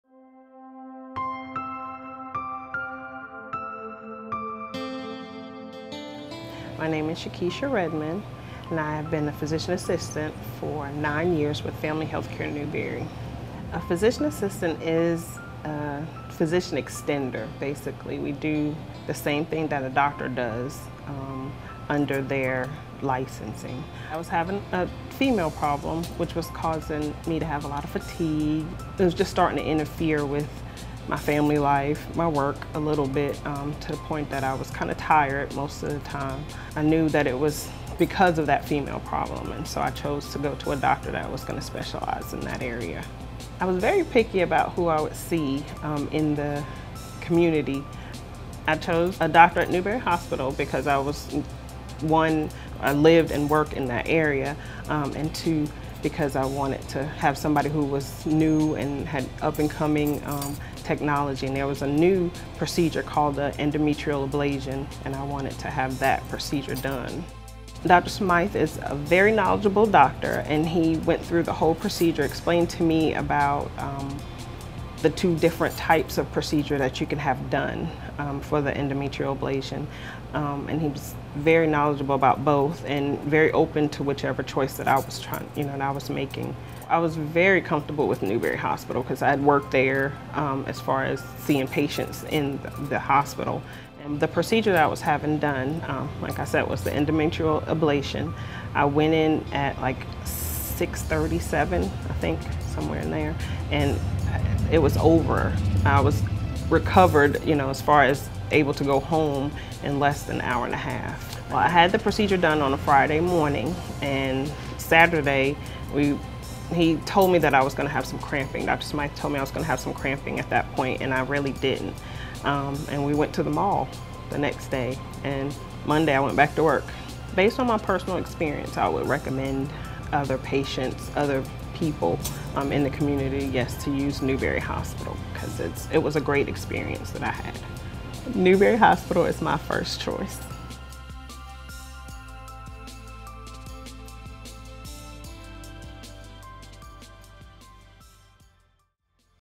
Women's Surgery at Newberry County Memorial Hospital
Newberry resident Shakisha Redmond made Newberry County Memorial Hospital her first choice when she needed women's surgery. Shakisha is a physician's assistant and knowledgable about how to choose the right hospital.